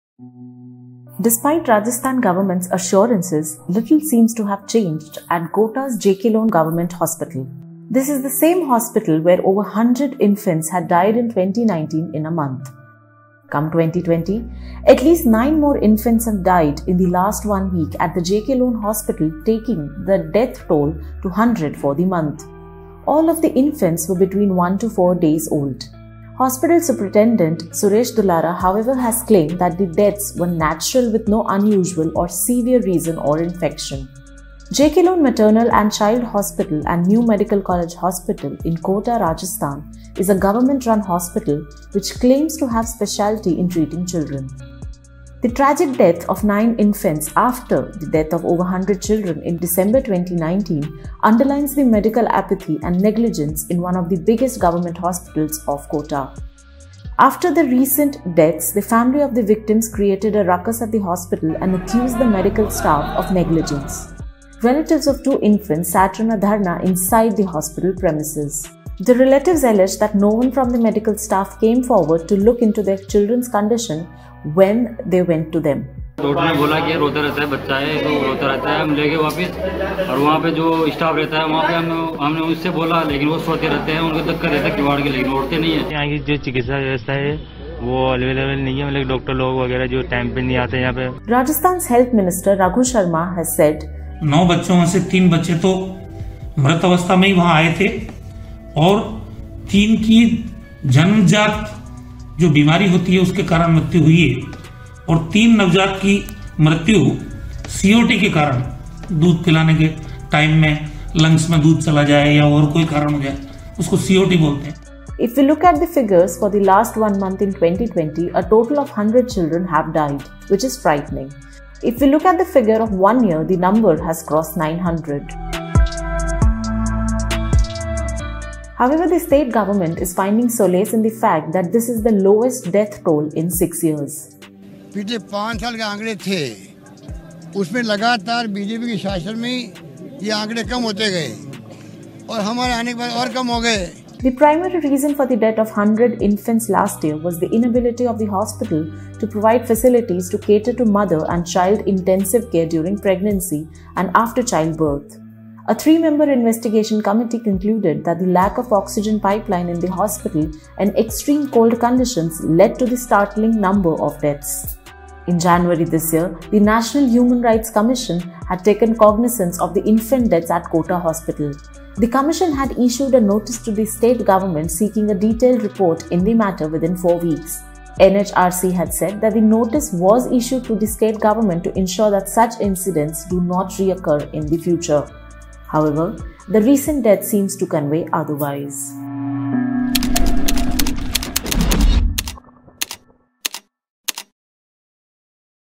Children's Death in Kota Hospital - A case of gross negligence
Despite Rajasthan government's assurances, little seems to have changed at Kota's JK Lone government hospital.  This is the same hospital where over 100 infants had died in 2019 in a month.

Last week in December at least nine more infants died at the hospital, taking the death toll to 100 for the month.  All of the infants were between 1-4 days old.

Hospital Superintendent Suresh Dulara however has claimed that the deaths were natural with no unusual or severe reason or infection. The parents however alleged that no one from the medical staff came forward to look into their children's condition when they went to them.

Watch the video to know more about the unfortunate incident.